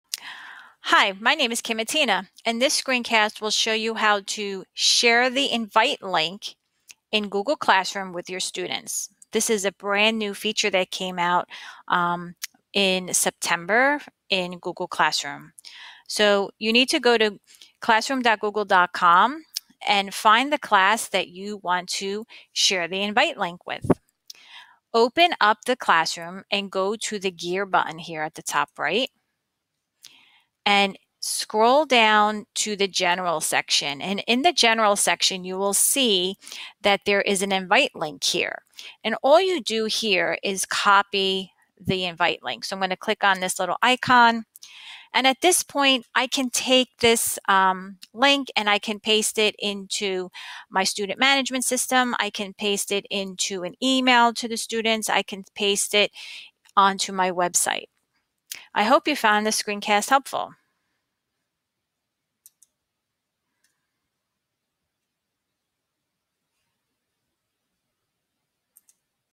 Invite students to Google Classroom using the invite link
This screen cast will demonstrate how to invite students to Google Classroom via the invite link.